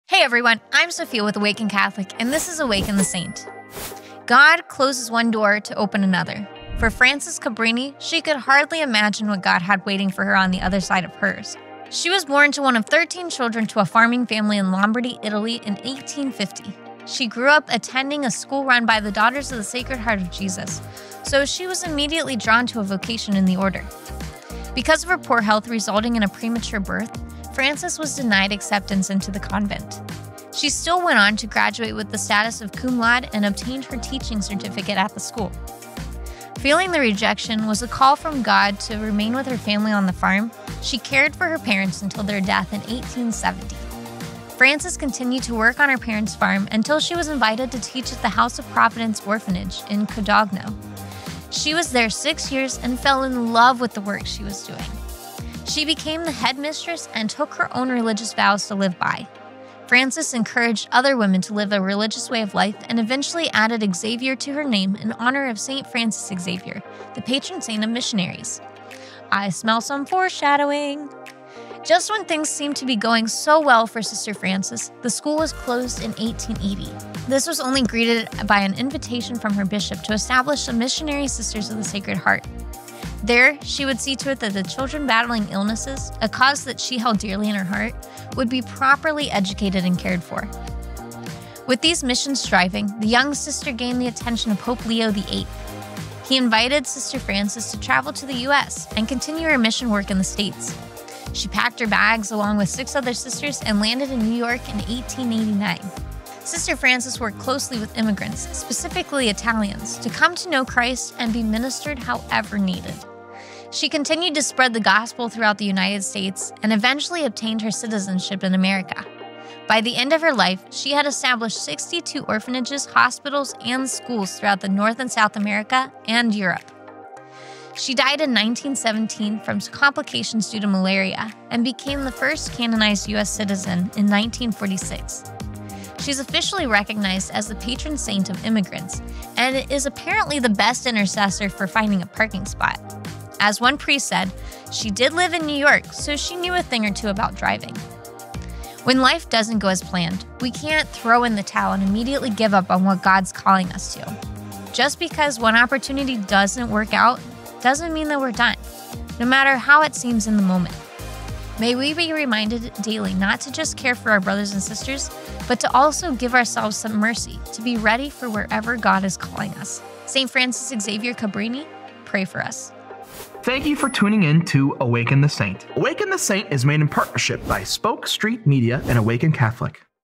St. Frances Xavier Cabrini: Patron Saint of Immigrants
Although initially denied entrance into religious life, Frances went on to found several orphanages, schools, and hospitals throughout America and Europe.

Saint Frances Xavier Cabrini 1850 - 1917. Feast day: November 13.

Patron Saint of: Immigrants

Show Host: Sophia Bauer